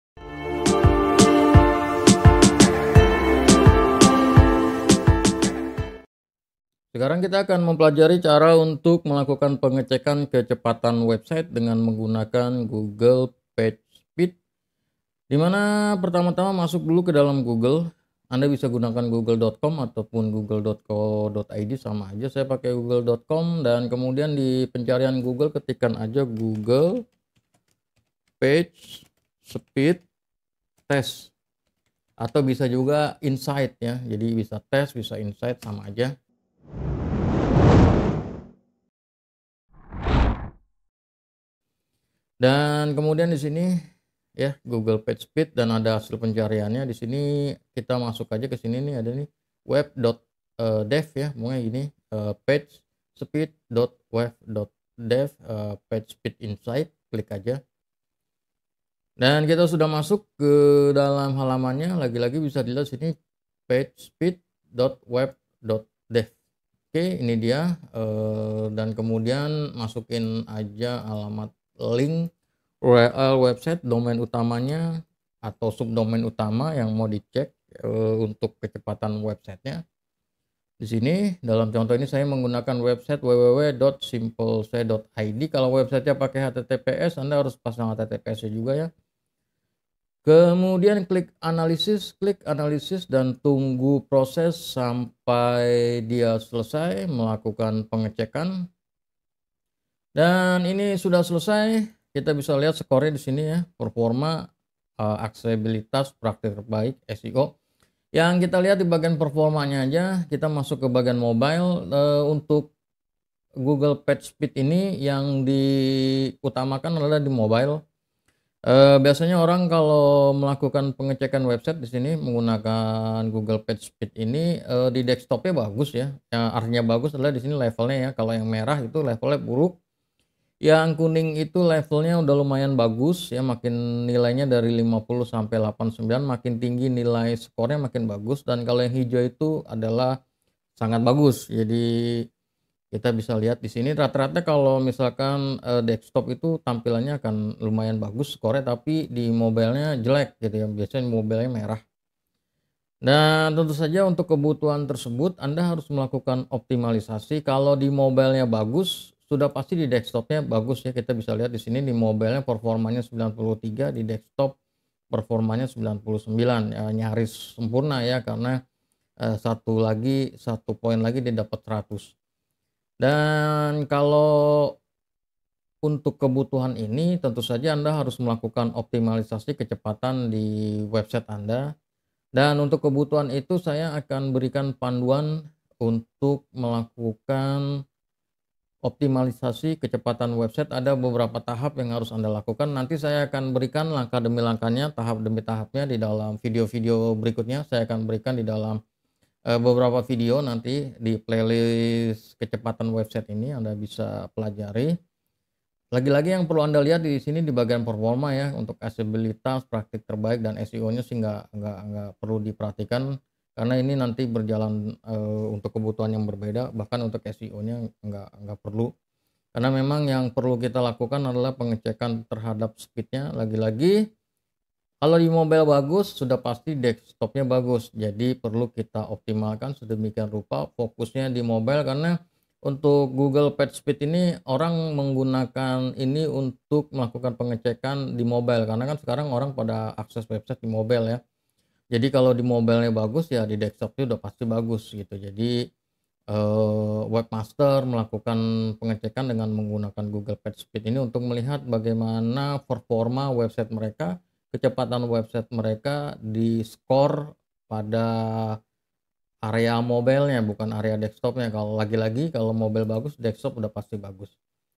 Cara Cek Kecepatan Website di Google - Optimalisasi Kecepatan Website
Panduan cara cek kecepatan website website di Google, bagi yang mau belajar cara optimalisasi kecepatan website bisa melihat pada playlist ini

Terima kasih,
Dukung Kami dengan membeli atau menggunakan jasa layanan yang kami berikan: